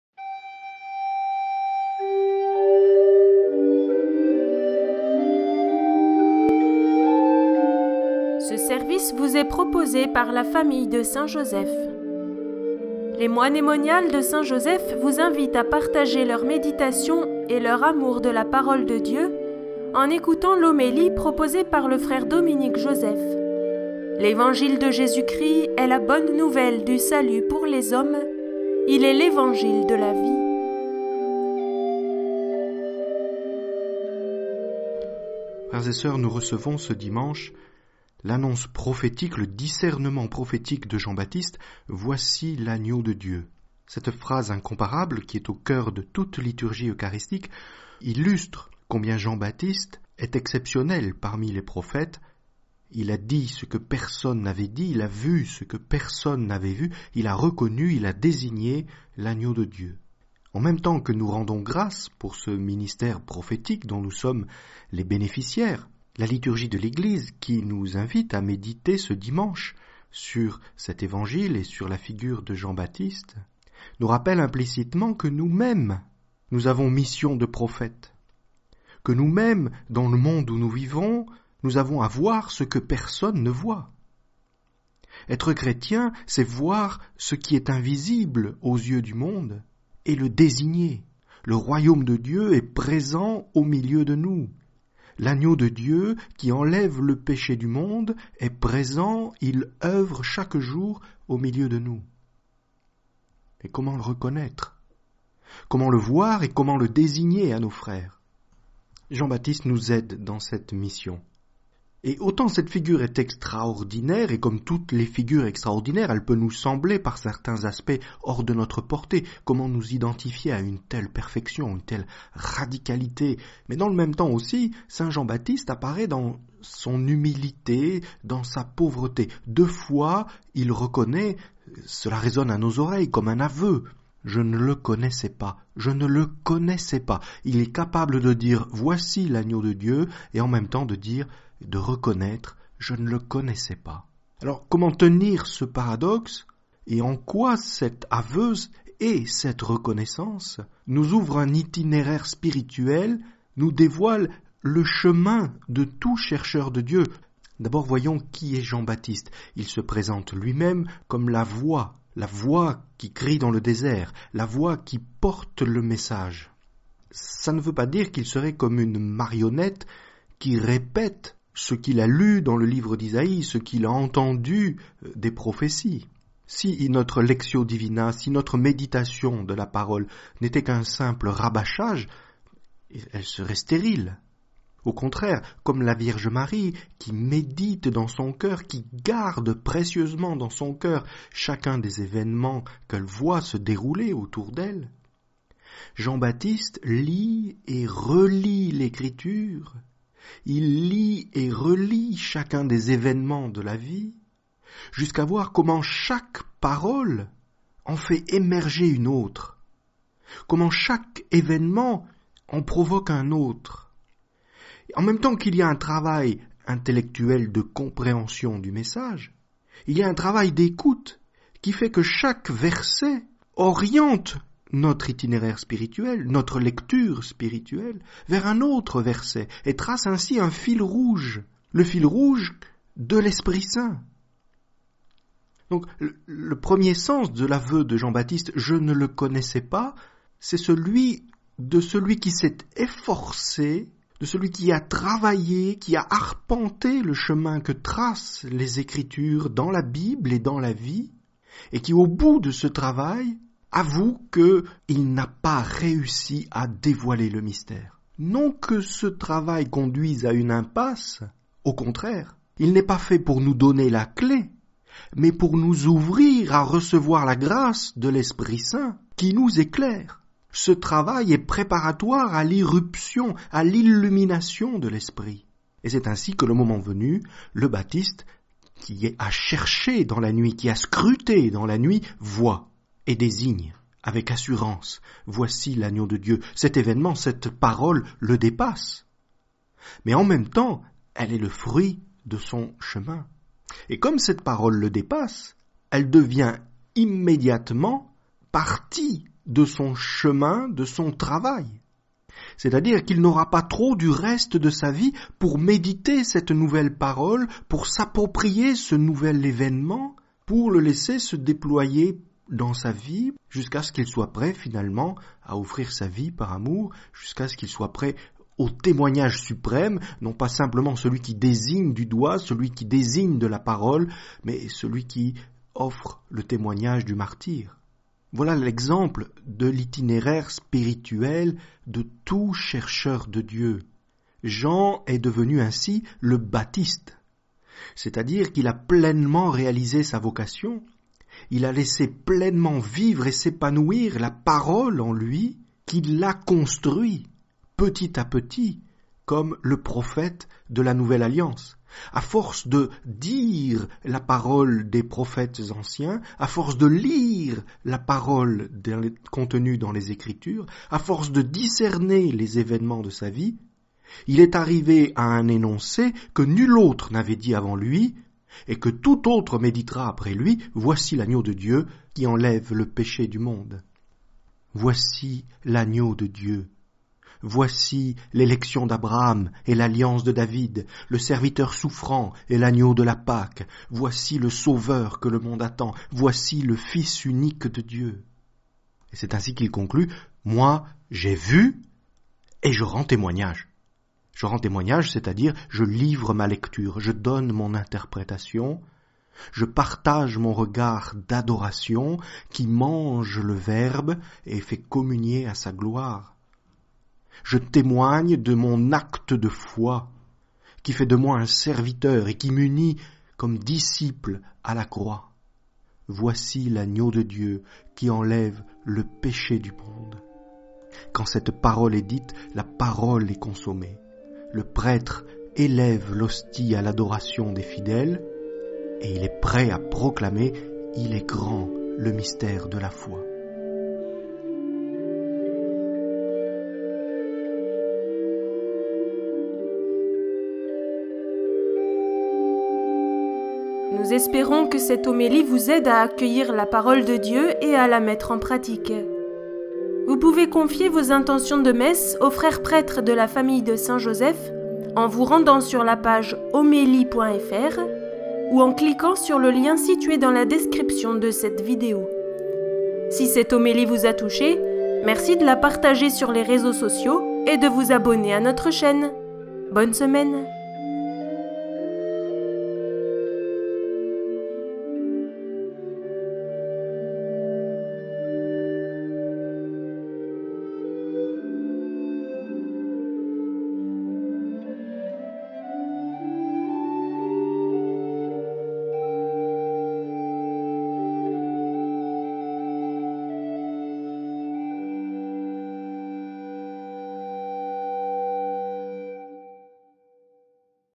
ITINÉRAIRE D’UN CHERCHEUR DE DIEU Homélie pour le 2e dimanche du T.O. | fr. Dominique Joseph
Confiez vos intentions de messe aux frères prêtres de la Famille de Saint Joseph

« Voici l’Agneau de Dieu, qui enlève le péché du monde » (Jn 1, 29-34)